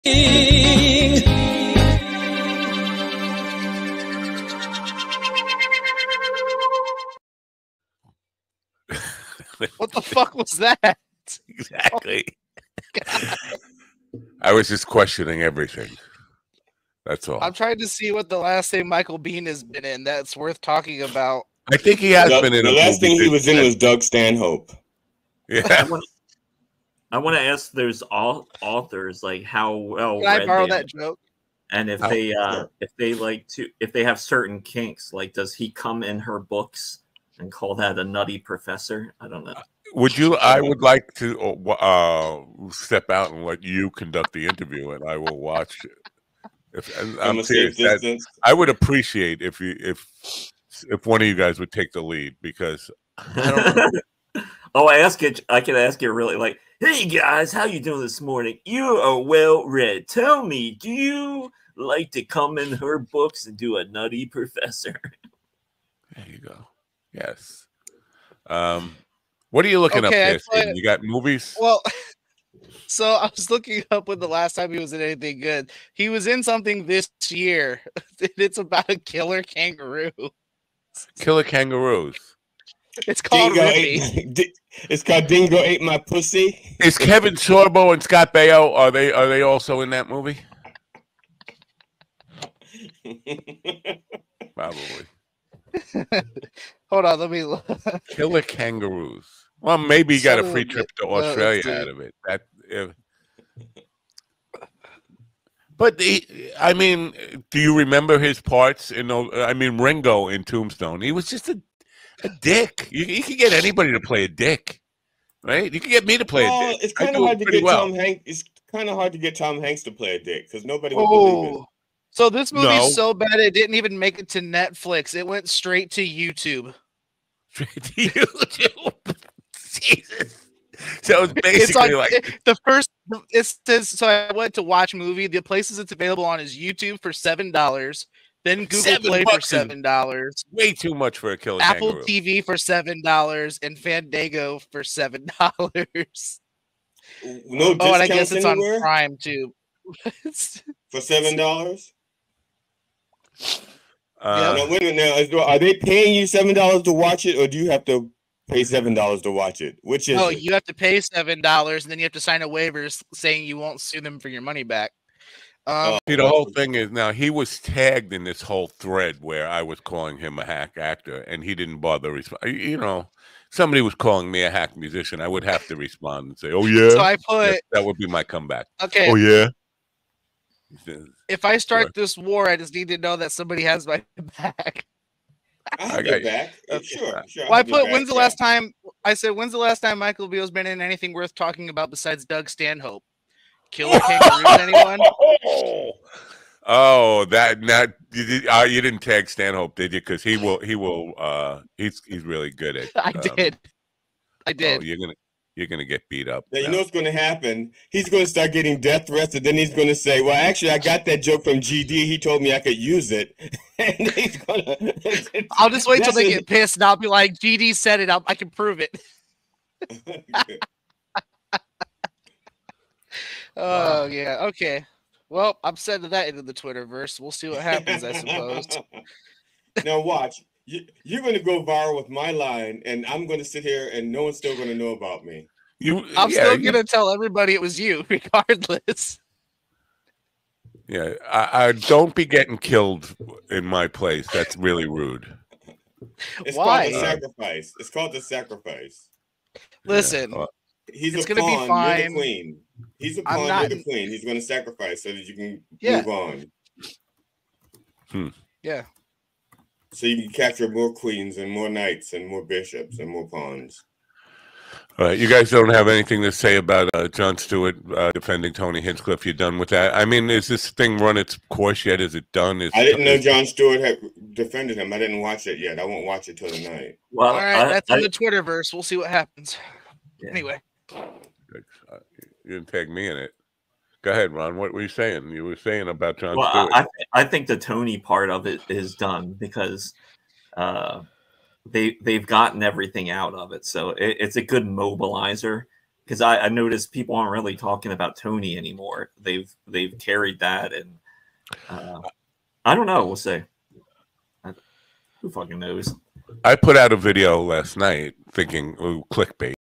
Is There Really a Movie About Killer Kangaroos
Discussing the movie career of the great Michael Beihn